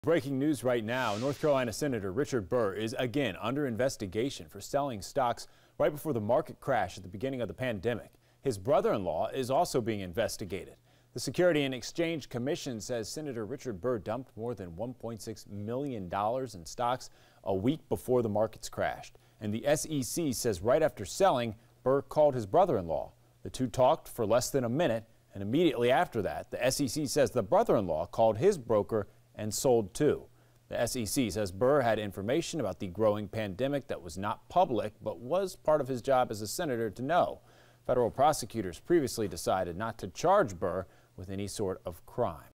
Report: Sen. Richard Burr, brother-in-law under investigation for possible insider trading
A new report from ProPublica indicates that there is an ongoing insider trading investigation into the trades of North Carolina U.S. Senator Richard Burr and his brother-in-law.

According to the ProPublica report, a new Securities and Exchange Commission (SEC) filing indicates that Sen. Burr had “material nonpublic information regarding the incoming economic impact of coronavirus.”